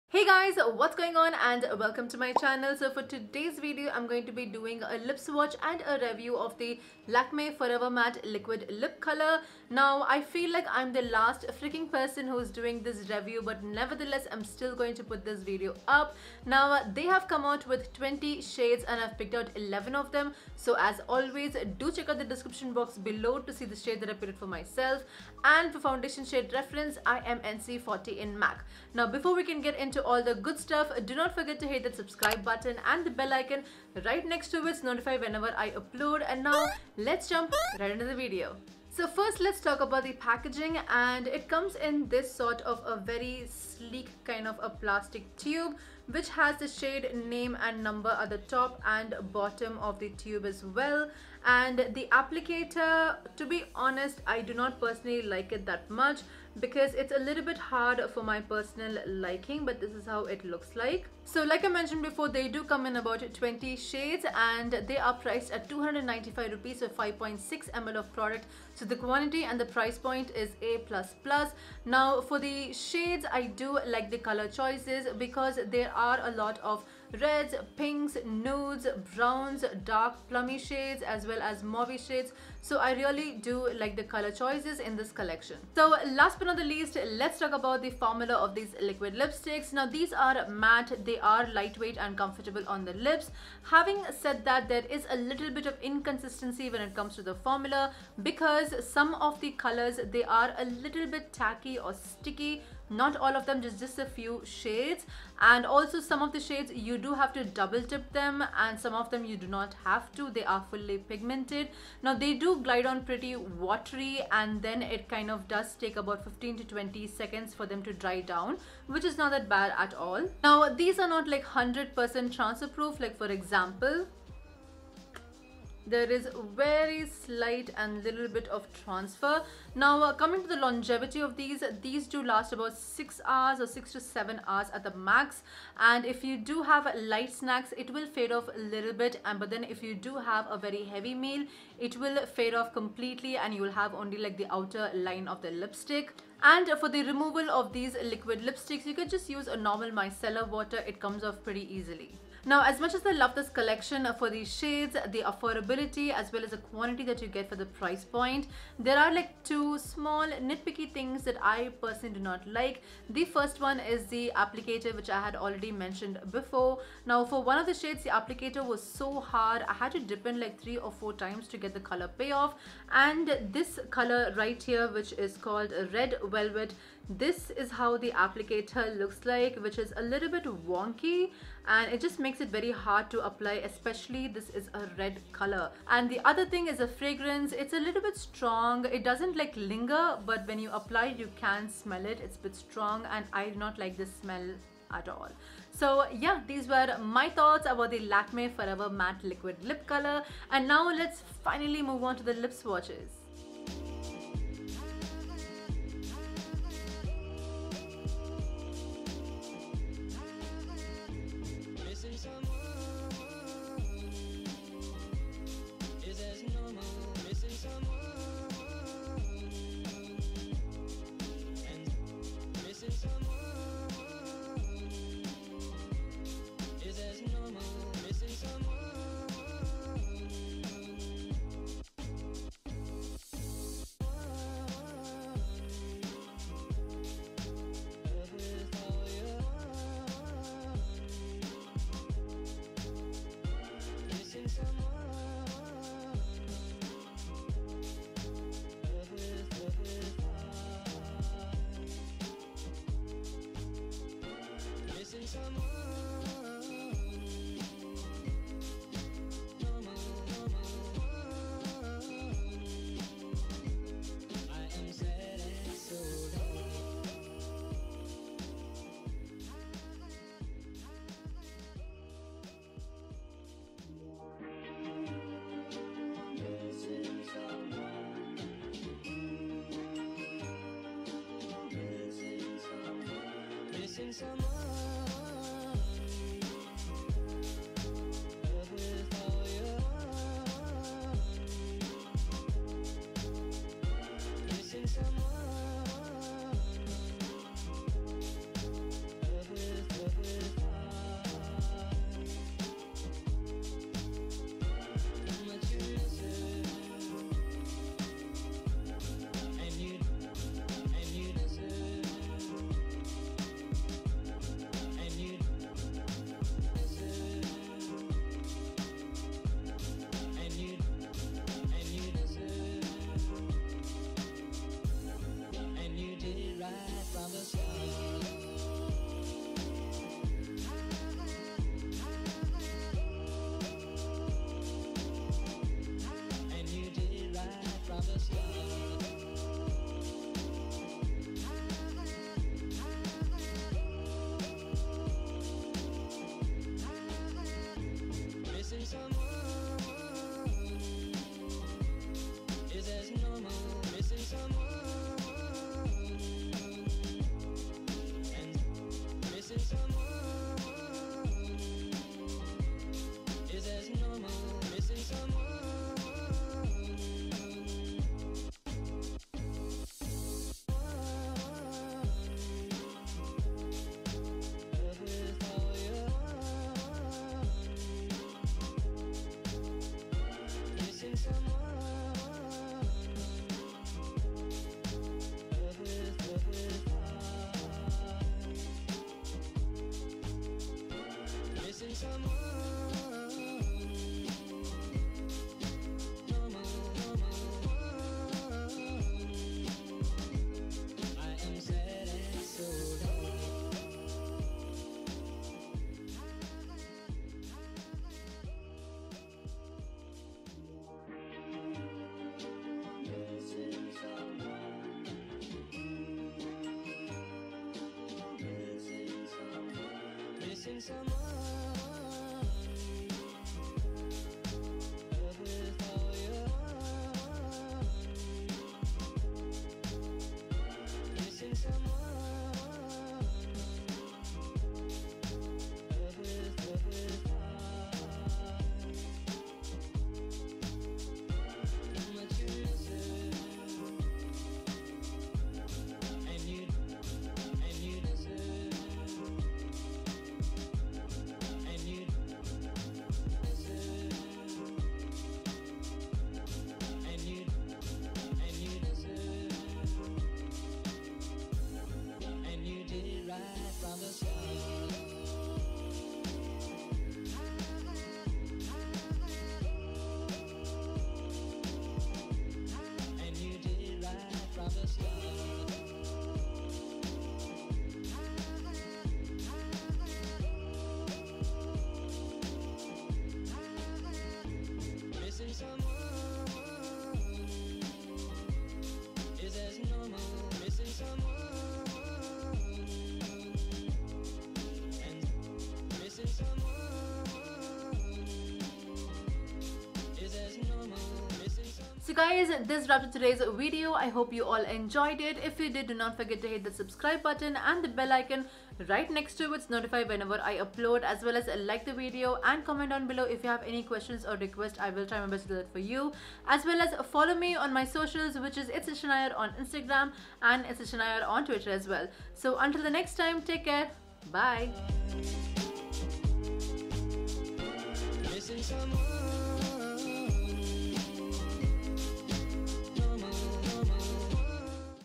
Lakme Forever Matte Liquid Lip Color Swatches & Review
Hey Guys,

Here is a video where I swatch & review 11 shades from Lakme Forever Matte Liquid Lip Color.

Lip Swatches starts at 03:23 😉

--- Products Used —

02 - Red Velvet

03 - Red Revival

08 - Nude Dream

09 - Pink Peach

12 - Pink Glam

13 - Nude Pink

14 - Mauve Party

17 - Nude Myth

18 - Brown Espresso

19 - Nude Twist

20 - Wine Touch

Music 🎵🎵🎵